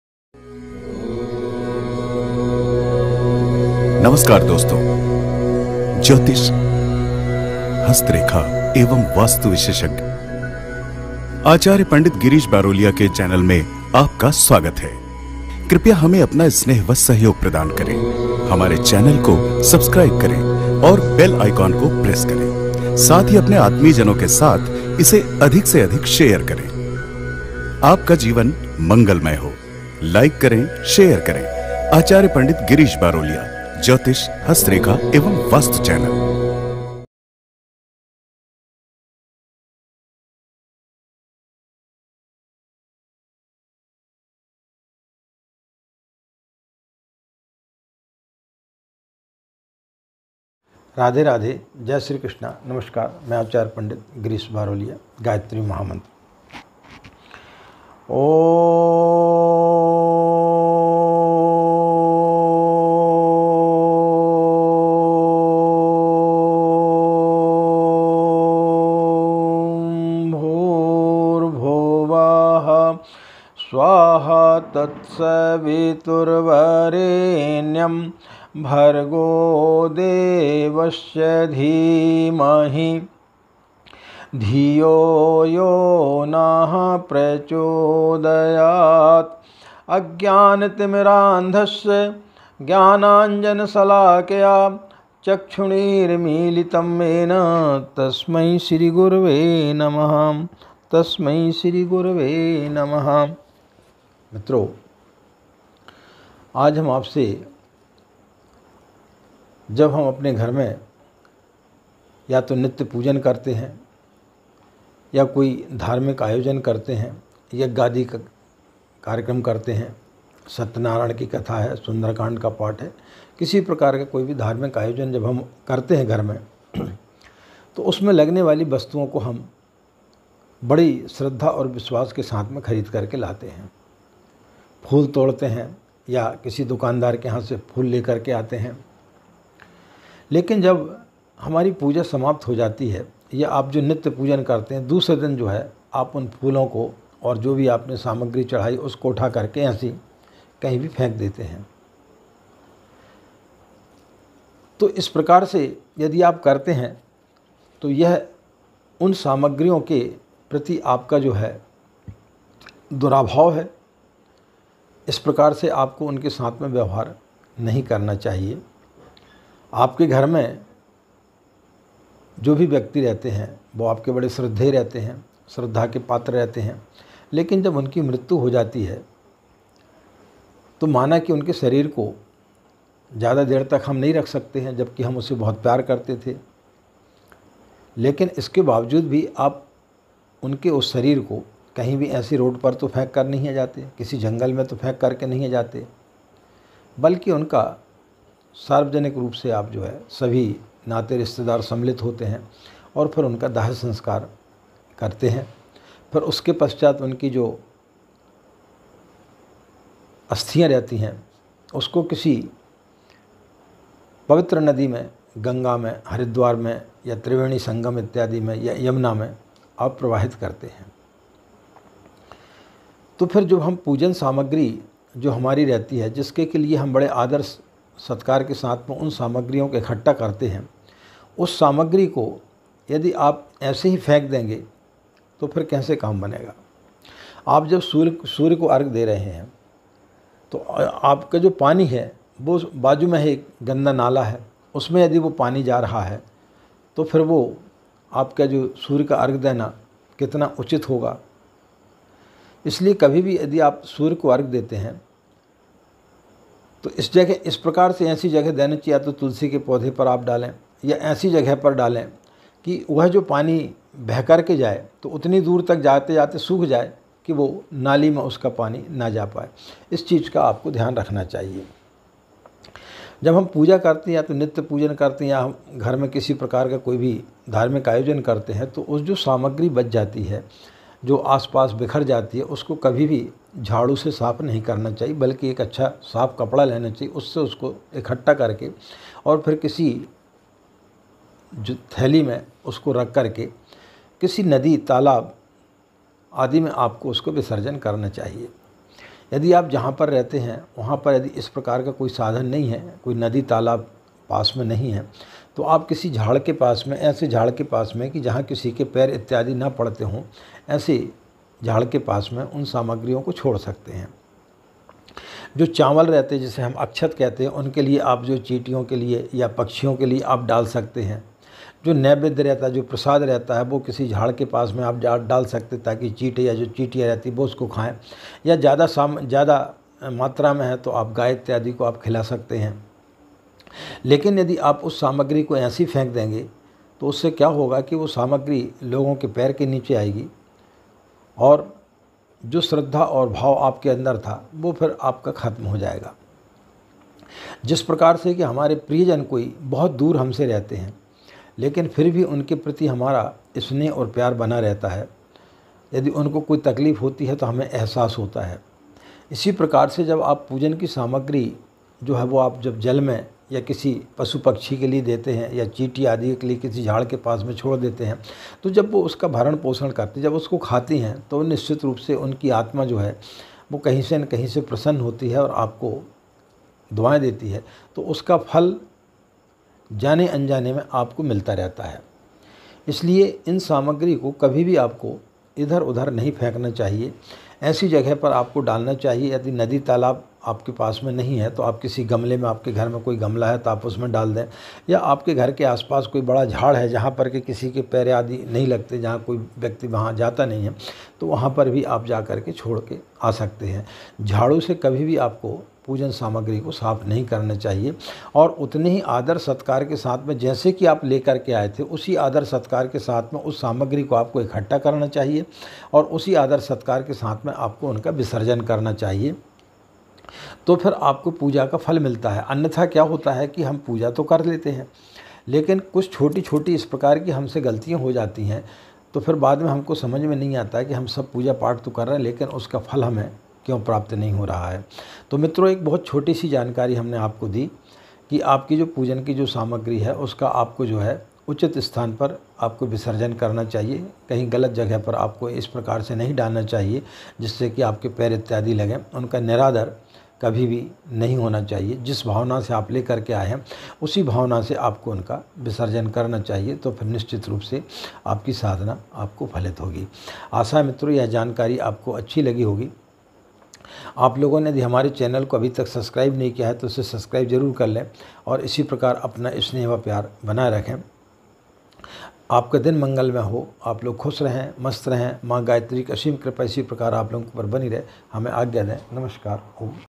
पूजा सामग्री का विसर्जन कहां करें||Where to immerse worship Material||Baroliya
मित्रों :-
हम घर में नित्य पूजन करते हैं ,या कोई धार्मिक आयोजन करते हैं ,उस में लगने वाली पूजन की जो सामग्री है उसे हमें इधर-उधर ऐसी जगह नहीं फेंकना चाहिए कि वह लोगों के पैरों पर पड़े|
 उसे किसी झाड़ के नीचे या बहते जल में प्रवाहित करने चाहिए इस सब की जानकारी इस वीडियो के माध्यम से हम आपके पास तक पहुंचा रहे हैं आशा है यह जानकारी आपके लिए अच्छी लगेगी और यदि आपको जानकारी अच्छी लगती है इसे अधिक से अधिक लोगों को शेयर जरूर करें
आपका अपना ही

 आचार्य पंडित  गिरीश बरोलिया|

Whatsapp For Appointment:-+91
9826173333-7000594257